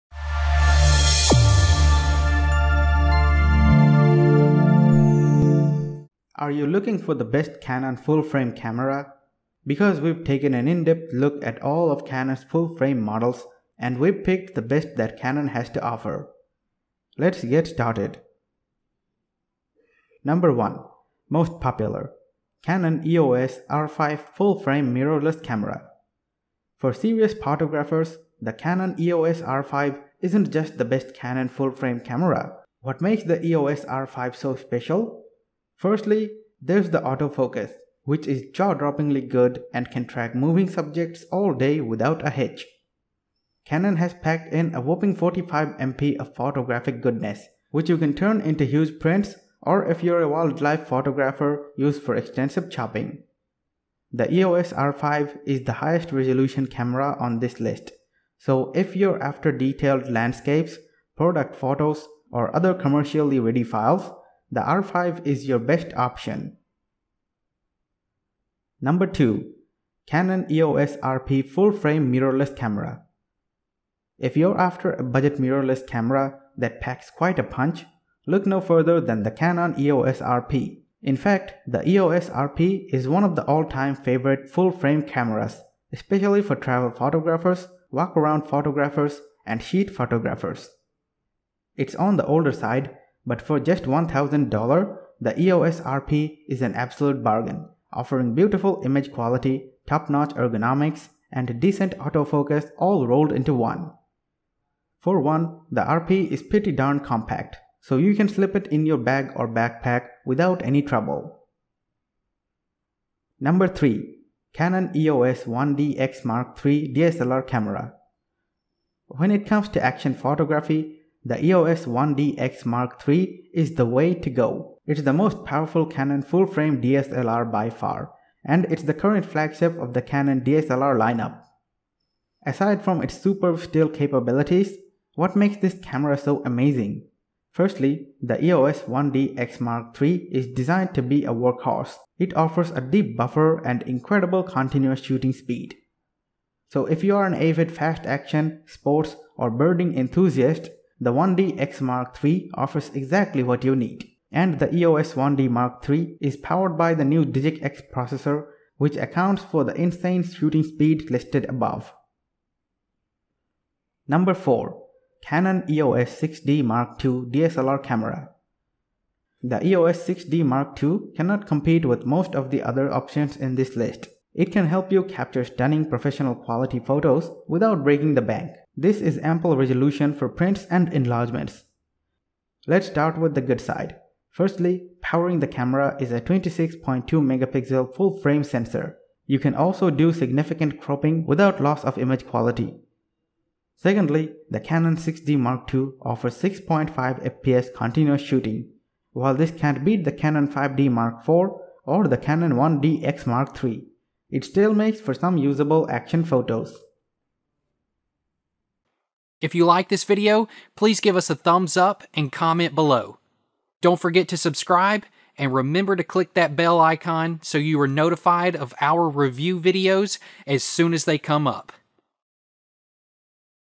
Best Canon Full Frame Camera for Traveling and Vlogging in 2024 - Top 4 Review
best canon full frame camera featured in this Video:
NO. 1. Canon EOS R5 Full-Frame Mirrorless Camera
NO. 2. Canon EOS RP Full-frame Mirrorless Camera
NO. 3. Canon EOS-1D X Mark III DSLR Camera
NO. 4. Canon EOS 6D Mark II DSLR Camera

🕝Timestamps🕝
0:00 - Introduction
0:20 - Canon EOS R5 Full-Frame Mirrorless Camera
1:08 - Canon EOS RP Full-frame Mirrorless Camera
1:52 - Canon EOS-1D X Mark III DSLR Camera
2:43 - Canon EOS 6D Mark II DSLR Camera

What is a canon full frame camera?
A canon full frame camera is a type of camera that offers wide dynamic range to preserve detail in highlights and shadows, low noise images straight from the camera and shallow depth of field for stunning portraits. They're built to deliver professional results with every shot you take.